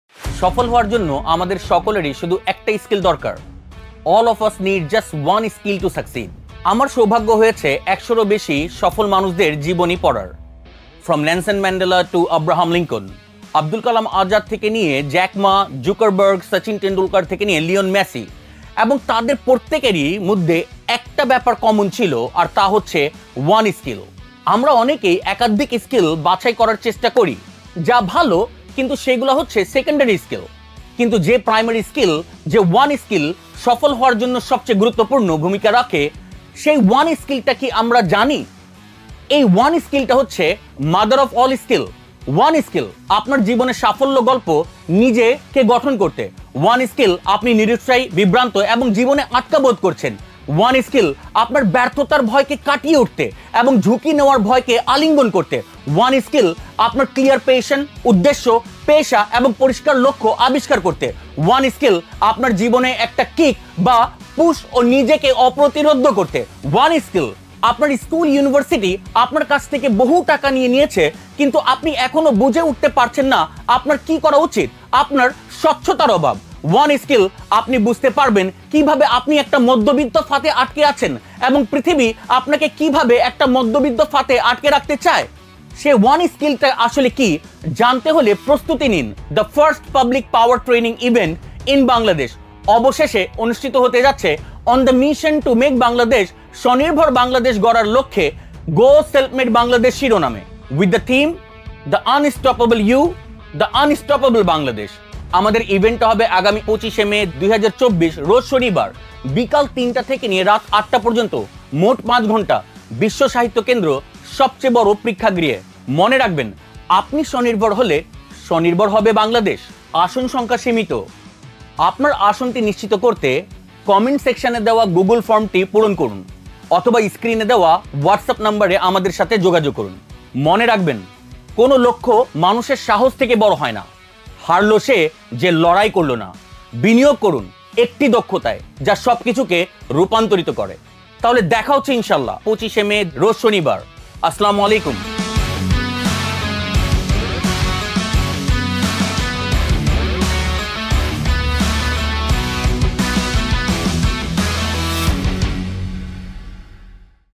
Promo of Public Power Training Event | Self Made Bangladesh | Chorka Communications
Upcoming Event : Public Power Training Event
Date : 25th May, 2024
Venue : Bissho Shahitta Kendra Auditorium
Client : Self Made Bangladesh
Event Partner : Chorka Communications